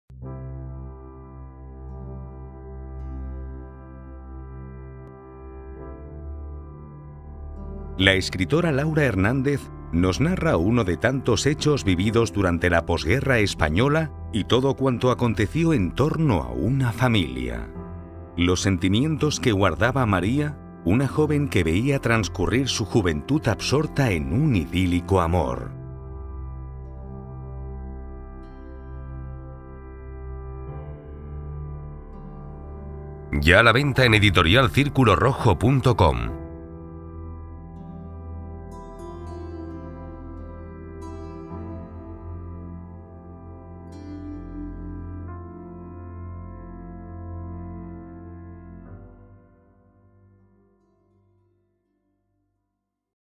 Una aguja en el diario (Booktrailer) - Editorial Círculo Rojo
“Una aguja en el diario” es una novela basada en uno de tantos hechos reales, conocidos a través de quienes los vivieron y que se desarrollaron en los años de la posguerra española (siglo xx) en el entorno rural. Las protagonistas sufrieron las consecuencias de la represión moral que se instauró durante esa época. María era una muchacha dedicada a servir a los “amos”, y que veía transcurrir su juventud absorta en un idílico amor que le causó más dolor que felicidad.